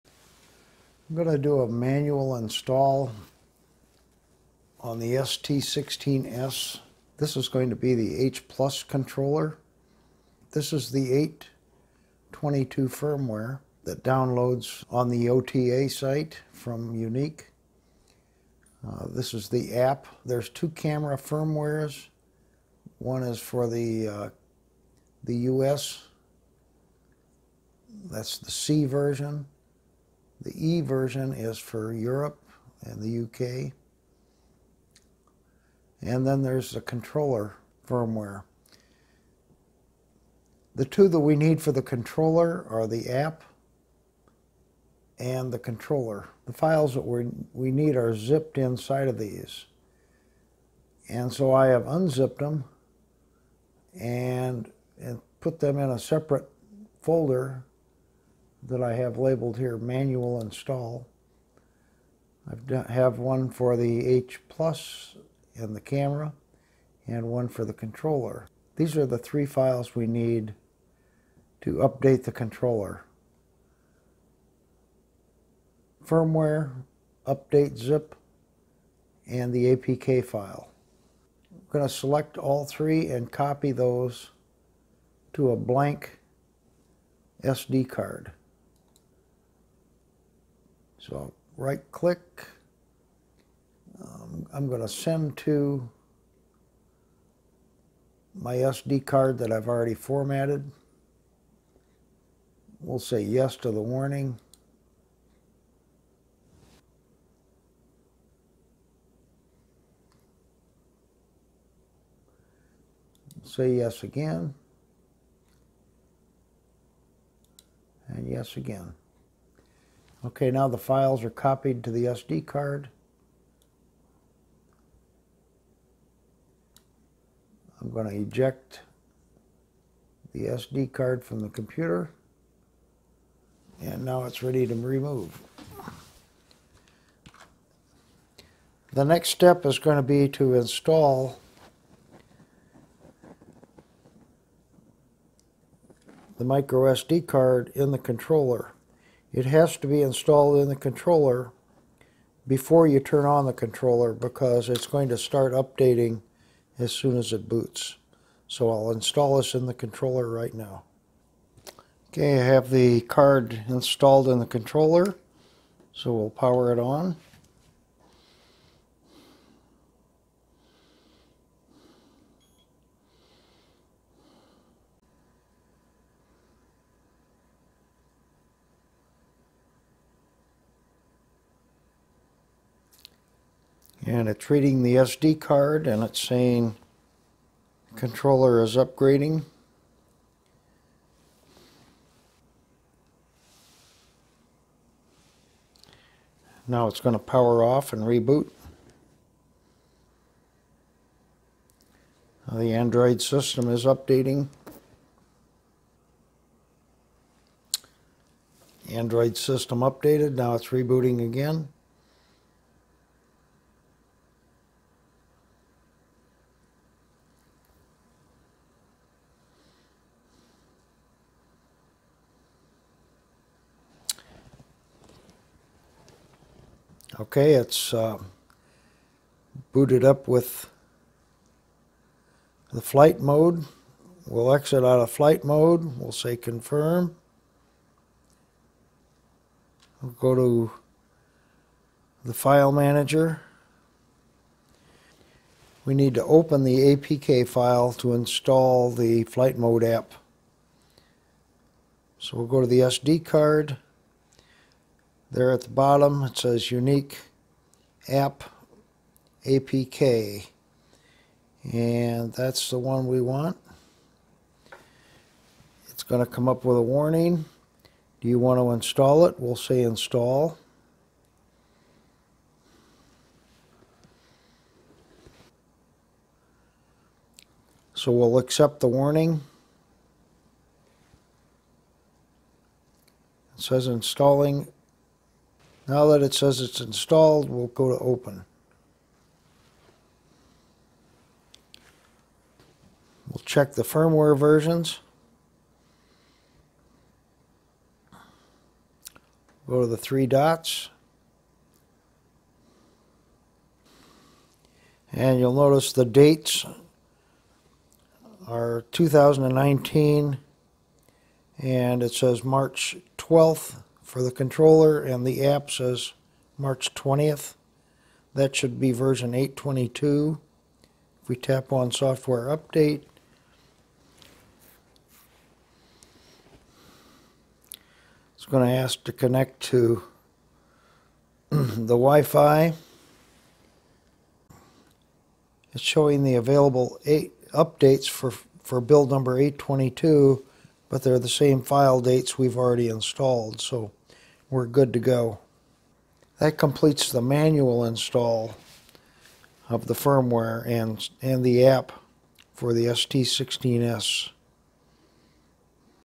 ST16s firmware manual Installation
Procedure to install the H Plus firmware on the controller without using the auto update. This method can be used to reinstall, upgrade or downgrade the firmware.

When installing the apk file you may have to go to the Android security settings and check the box "Unknown Sources". Then go back to File Manager and select the apk file and then select install.

The Yuneec server is currently offline so the automatic update is not possible.